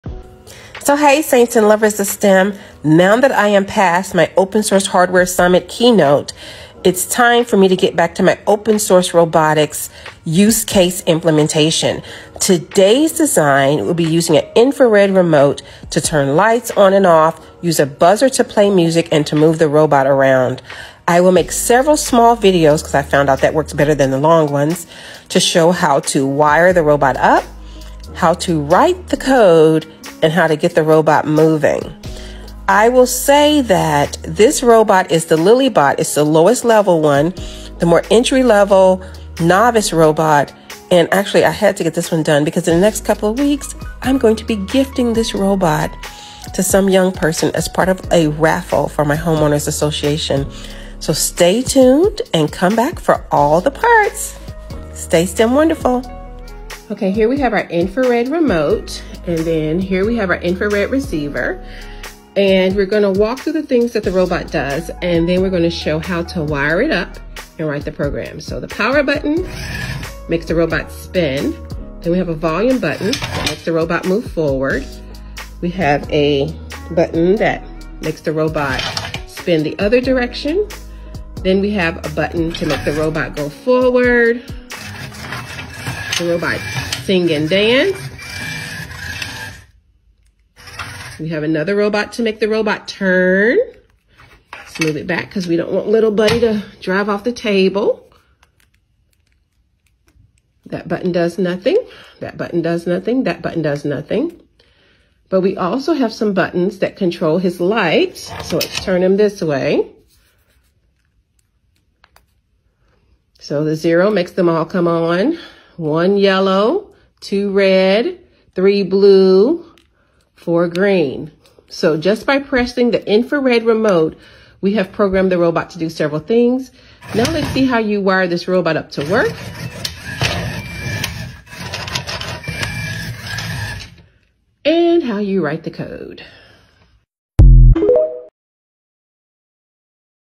Lily∞Bot: IR Remote Control Part I
This video demonstrates how to use an IR Remote and Receiver to control the robot to move, dance, sing, and light up.

Videos about engineering education, robotics education and diversifying STEM.
Carlotta A. Berry, PhD NoireSTEMinist Bringing STEM 2 to people & people 2 STEM to diversify STEM